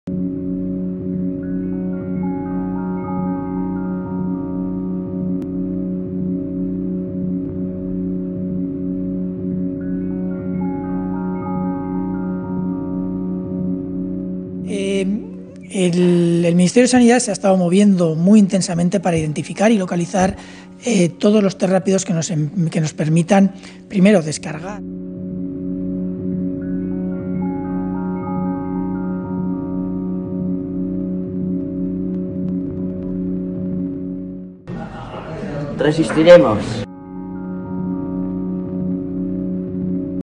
西班牙马德里养老院1065名老人去世
【西班牙马德里养老院1065名老人去世# 检测不足无法判断是否因为新冠】3月26日，据西班牙卫生部调查报告，3月疫情暴发后马德里各养老院共1065名老人逝世。卫生部官员称由于缺乏检测，无法确定因新冠肺炎死亡比例，但补充说去年同期马德里去世老人约为200人。目前马德里死亡病例占全国51%，养老院为疫情暴发点。